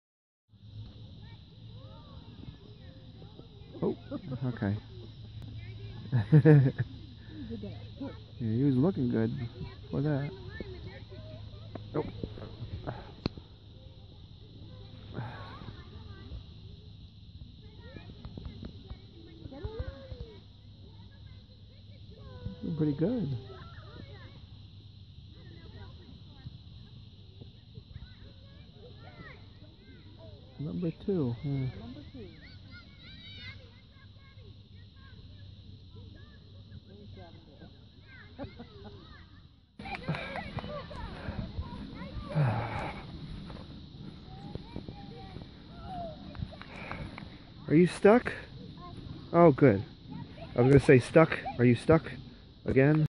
863 - Ethan Kicking a Soccer Ball (Gets Foot Stuck in Net)
Ethan was kicking a soccer ball and looking good. ... Of course, I did catch him getting his foot stuck in the net ... again! (I didn't catch the first time. lol) ...